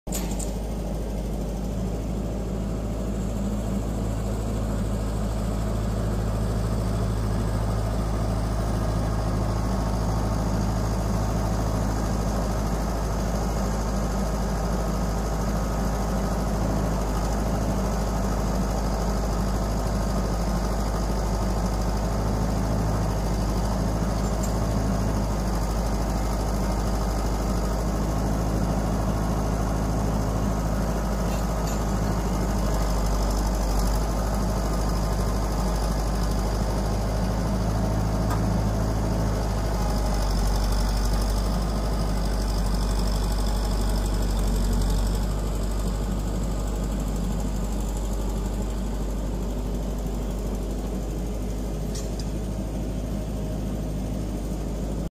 32" log grapple
removing logs from my pond